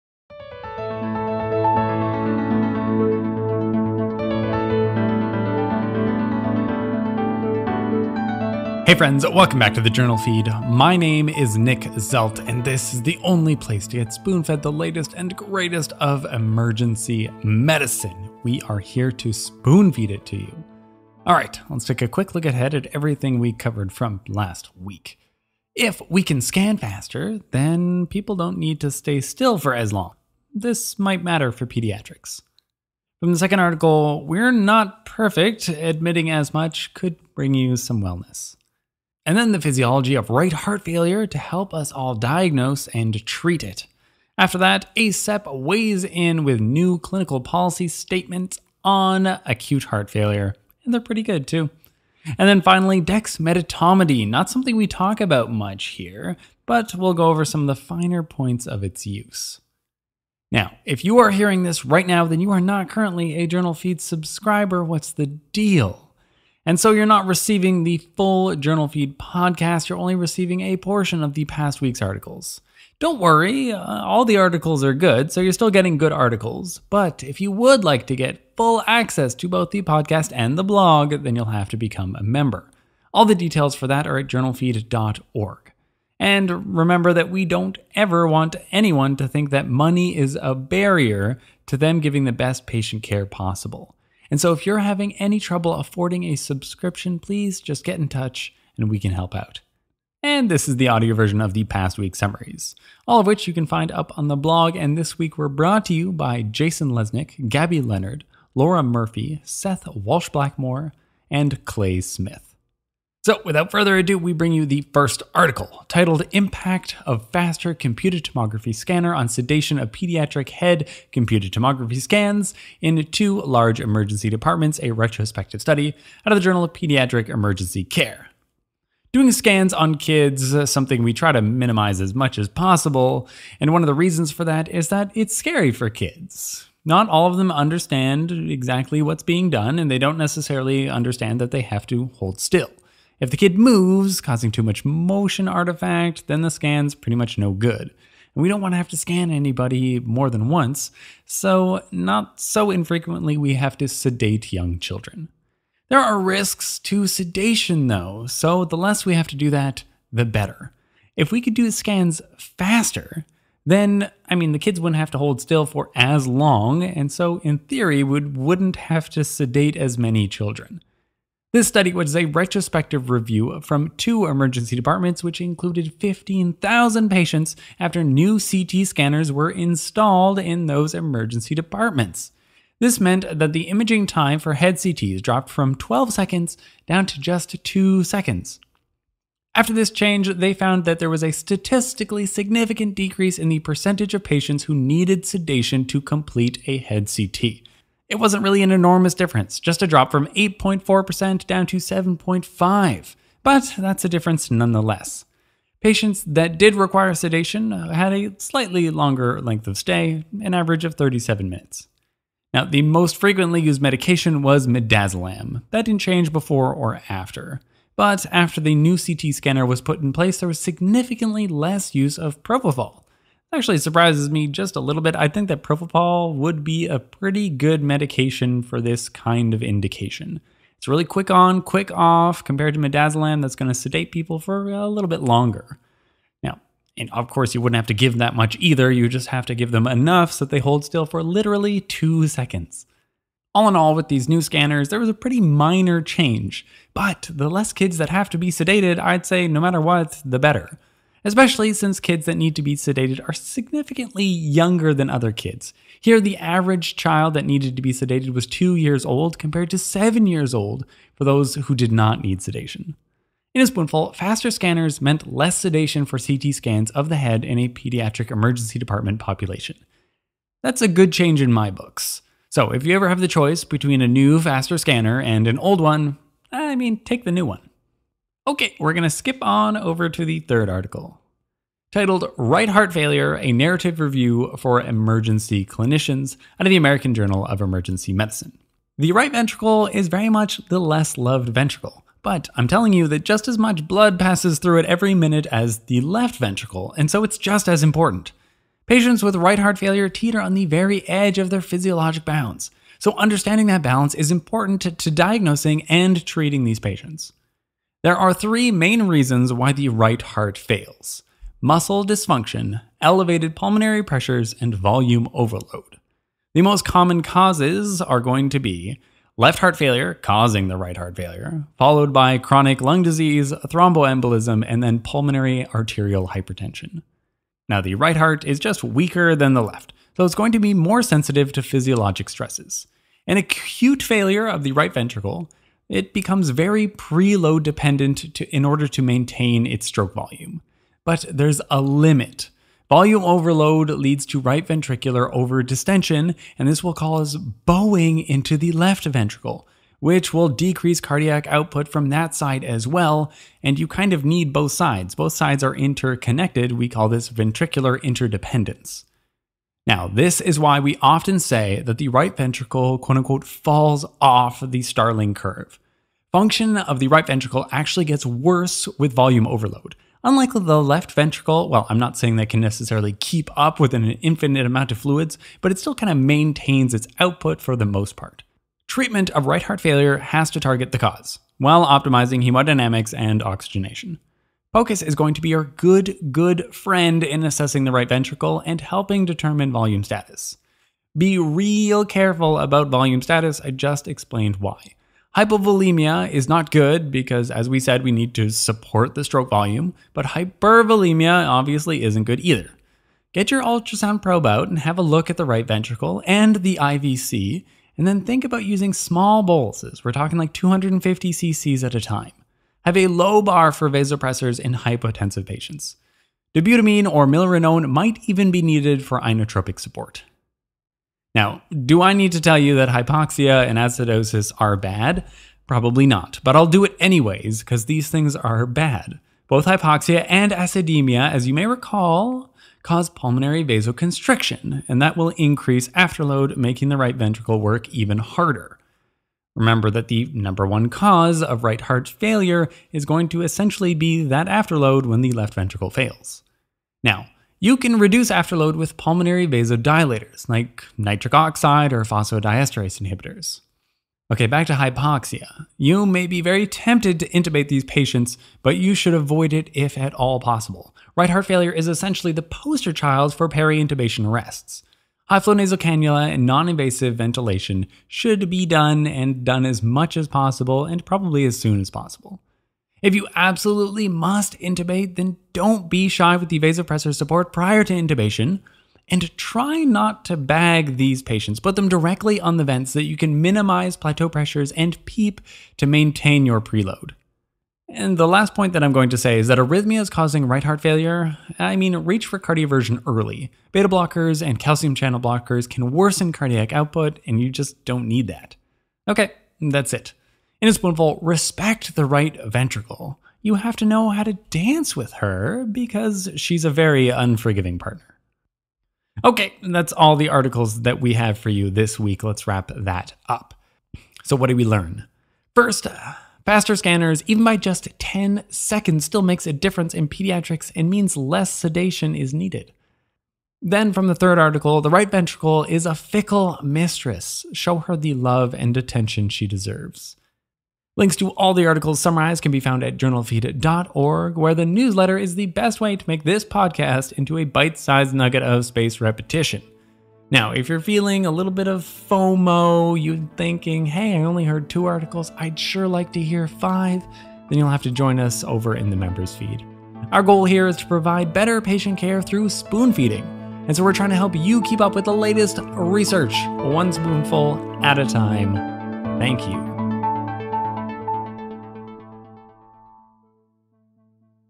Go Scanner Go | How PEs Kill
The JournalFeed podcast for the week of Nov 14-18, 2022.

These are summaries from just 2 of the 5 article we cover every week! For access to more, please visit JournalFeed.org for details about becoming a member.

Go Scanner Go Spoon Feed

Newer CT scanners led to faster head CT by about 10 seconds, and this decreased the number of pediatric patients who required sedation.

How PEs Kill Spoon Feed

Decompensated right heart failure (RHF) increases morbidity and mortality, but it can be challenging to diagnose and treat. This review summarizes the unique physiology of RHF and treatment implications.